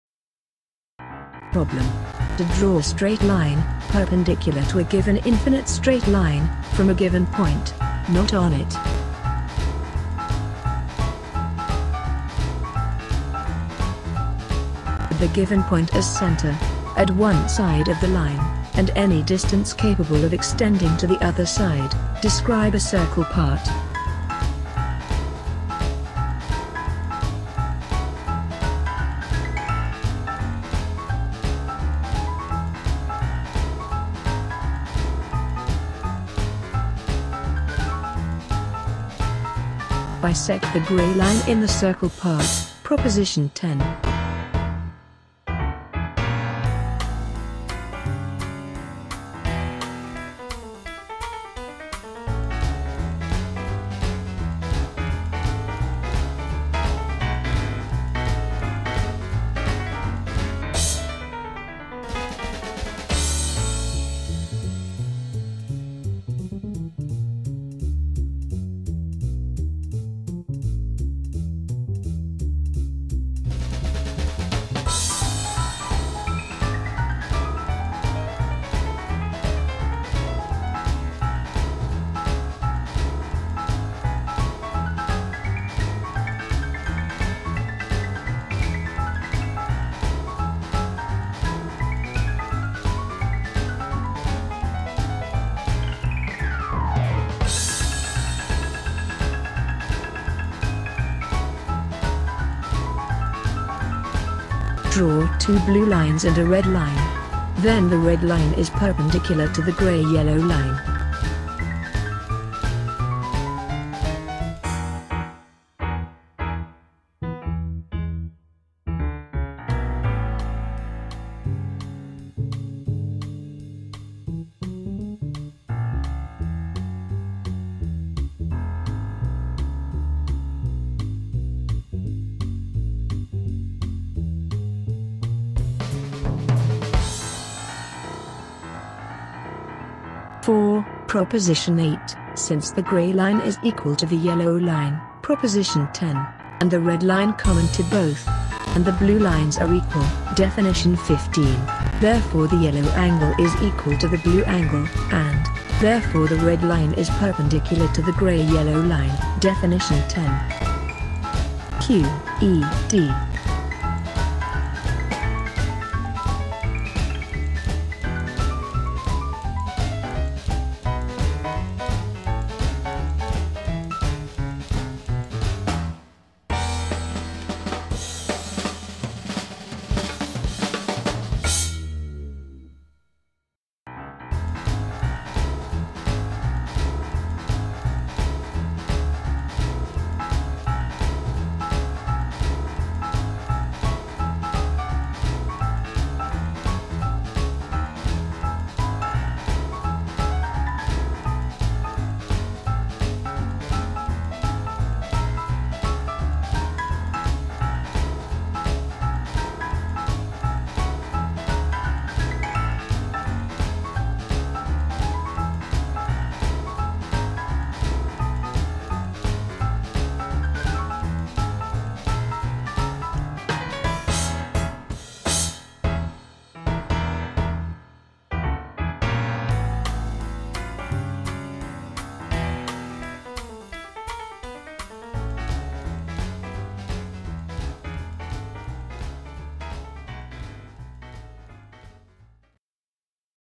The Elements of Euclid. Book 1, proposition 12.
The Elements of Euclid.
Book 1, proposition 12: To draw a straight line perpendicular to a given infinite straight line from a given point not on it.

Animation based on the book:
The first six books of the Elements of Euclid, in which coloured diagrams and symbols are used instead of letters by Euclid; Byrne, Oliver
Publisher: London, William Pickering
Publication date: 1847